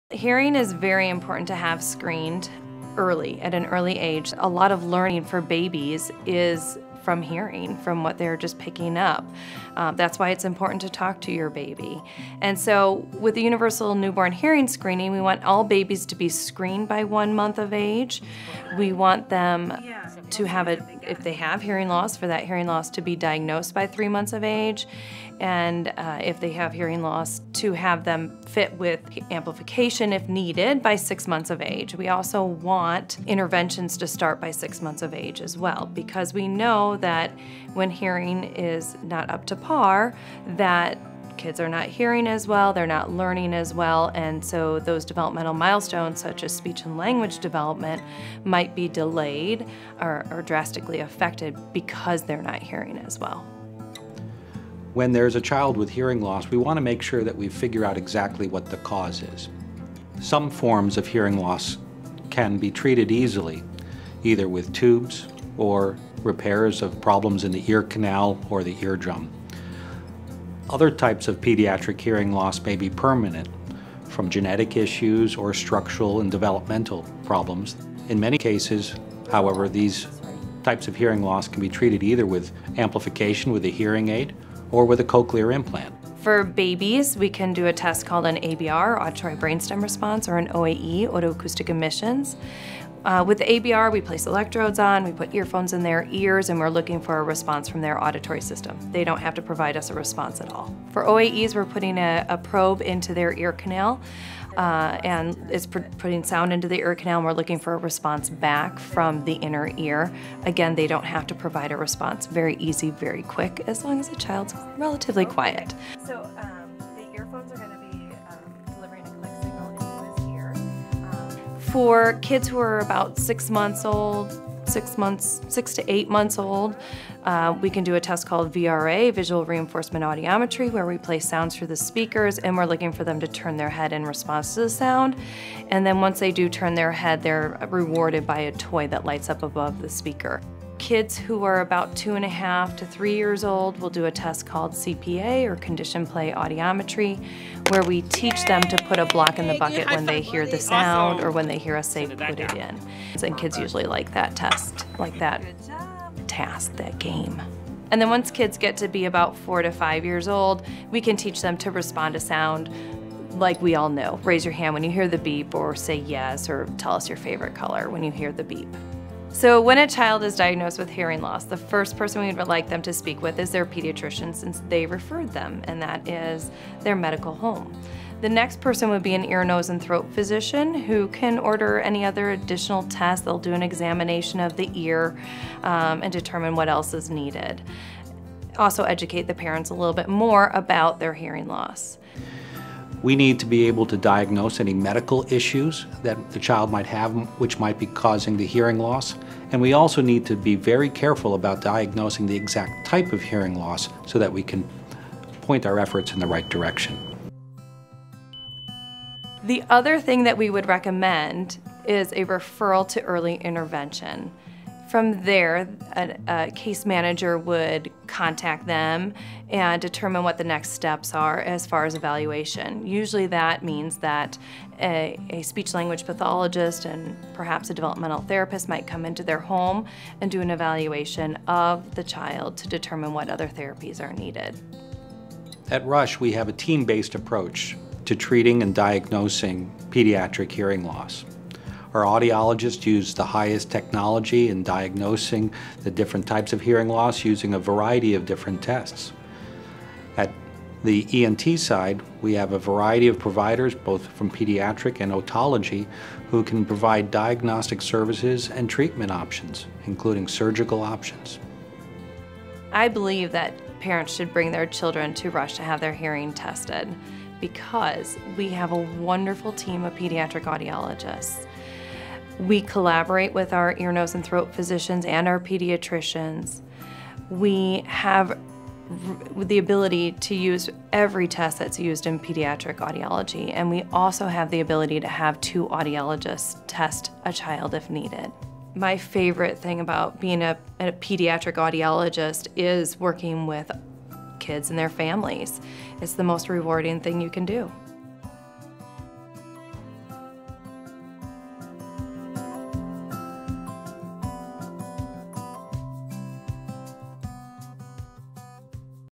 Testing Children's Hearing
Why it's important to test children's hearing at an early age.